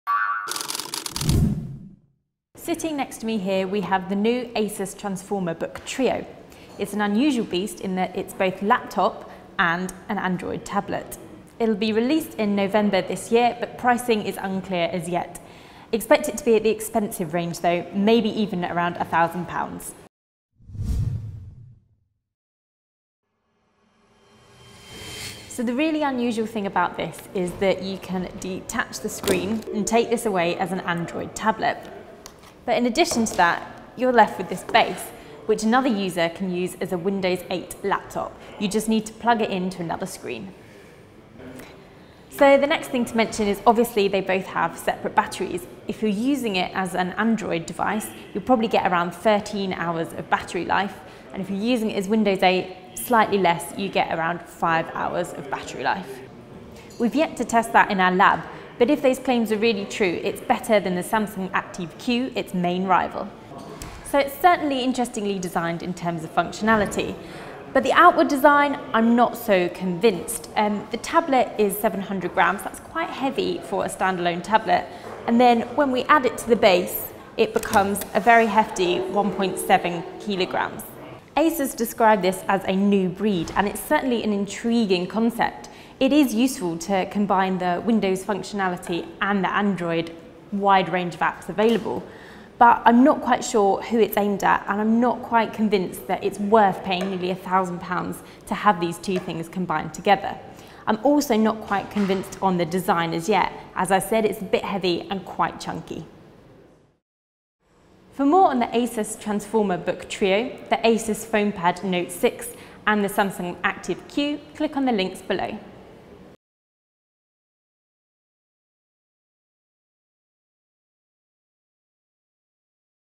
Asus Transformer Book Trio - Which? first look from IFA 2013
You've no doubt heard of a two-in-one device such as a tablet with a detachable laptop-style keyboard. But with the Trio Asus has gone one step further, this is a three-in-one combo designed to serve your every computing need.

The Transform Book Trio blends an Android tablet, Windows 8 laptop and PC in one unique body quite unlike anything we've ever seen before. The only thing remotely in the same realm as the Trio is the Samsung ATIV Q -- which runs both Android and Windows 8.